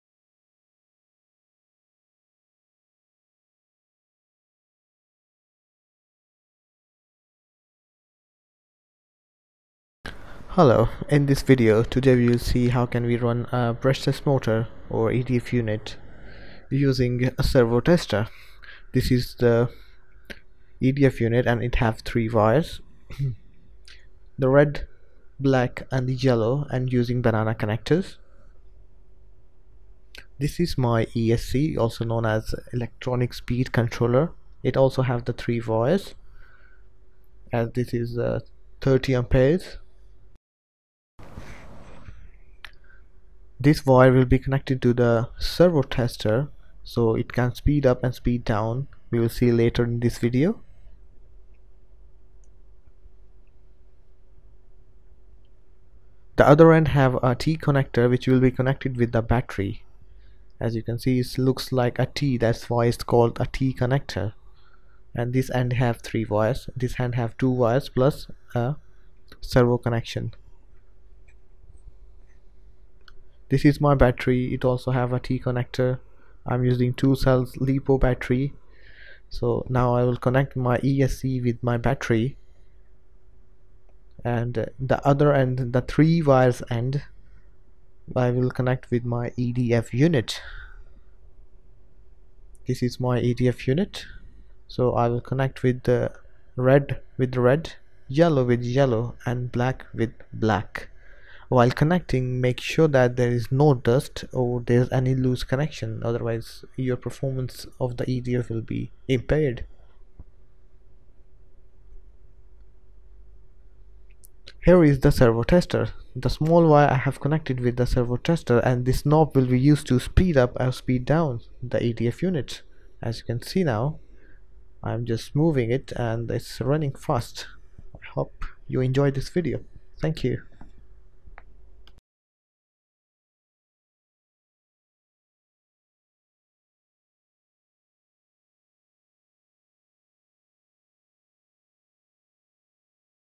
How to Run EDF Brushless Motor without Radio Transmitter Rx Tx RC using Brushless Motor Driver
In this video, we will see How can we Run and test the EDF unit brushless motor ( having 3 Wires ) without using remote transmitter and receiver.

In this video :

64mm EDF 3200kv Motor
30A Top RC Hobby ESC ( Brushless Motor Driver )
Servo Tester
2S Li-Po Battery with T connector